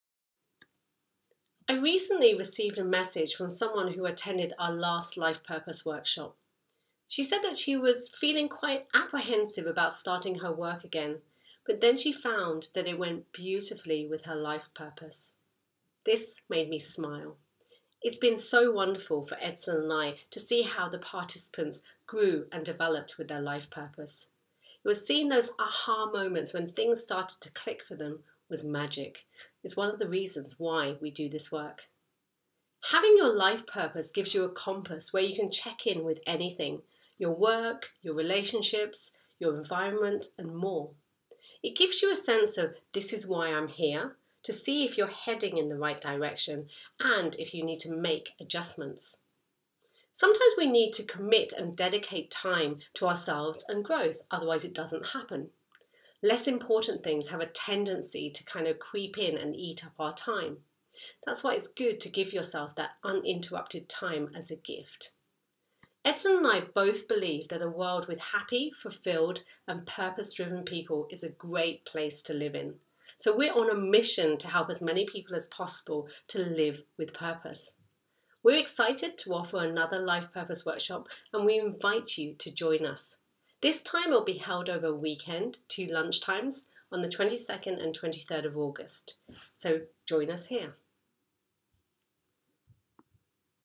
#399 Creating Your Life Purpose
I recently received a message from someone who attended our last Life Purpose Workshop. She said that she was quite apprehensive about starting her work again but then she found that it went beautifully with her life purpose.